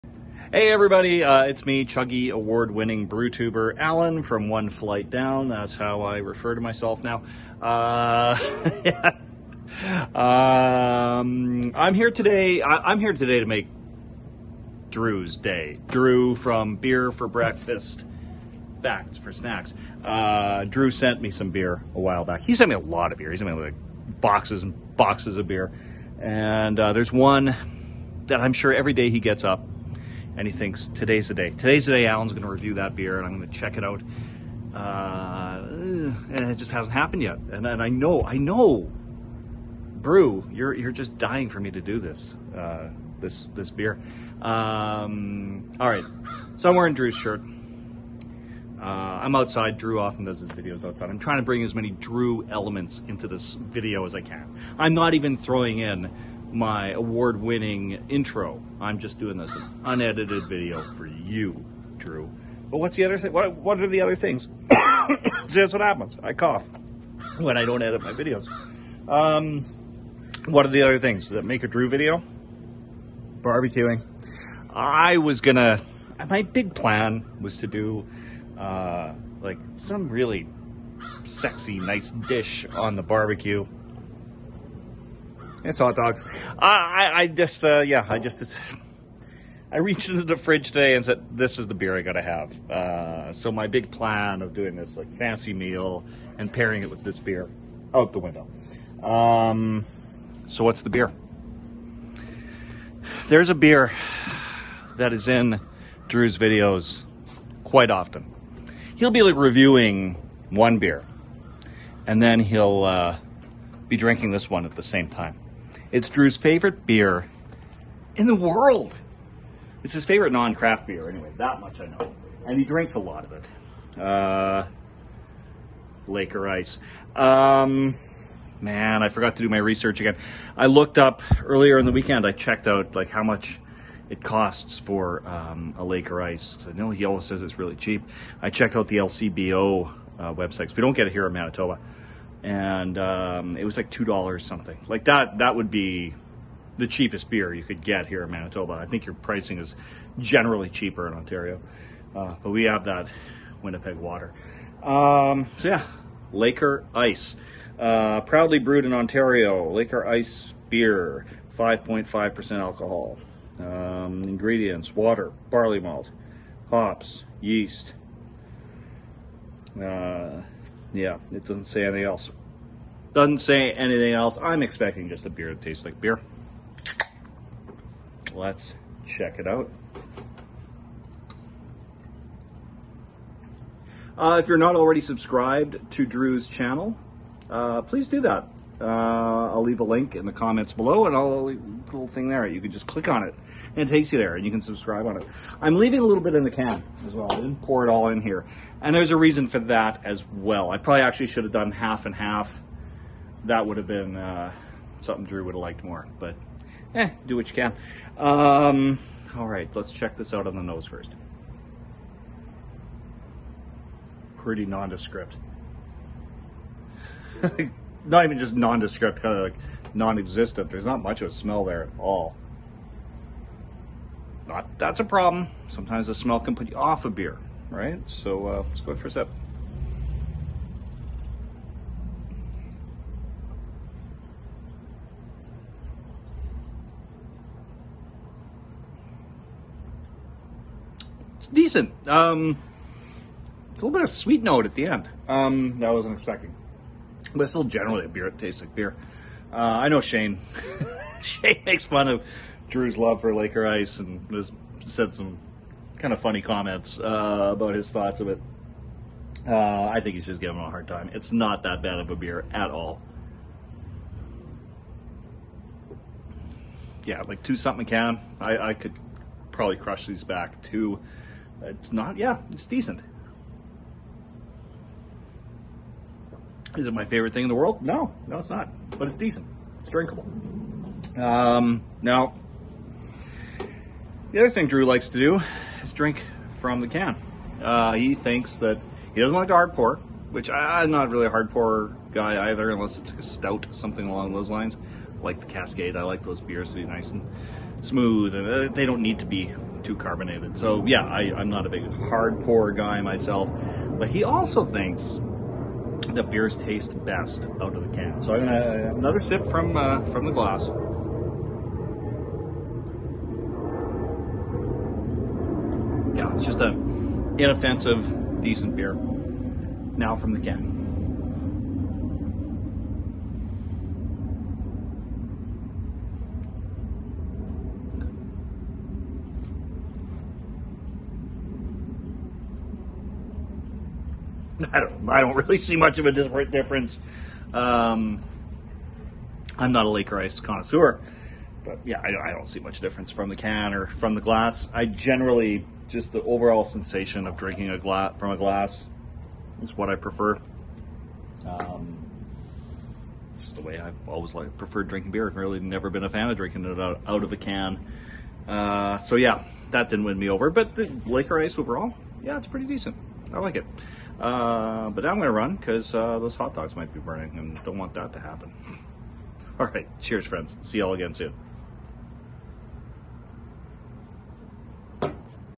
Craft Beer Geek Tries Laker Ice
Episode 176 - Alan tries a very special beer from Brew at @BeerforBreakfastFactsforSnacks

Featured beer:
Laker Ice
Waterloo Brewing

Cheers!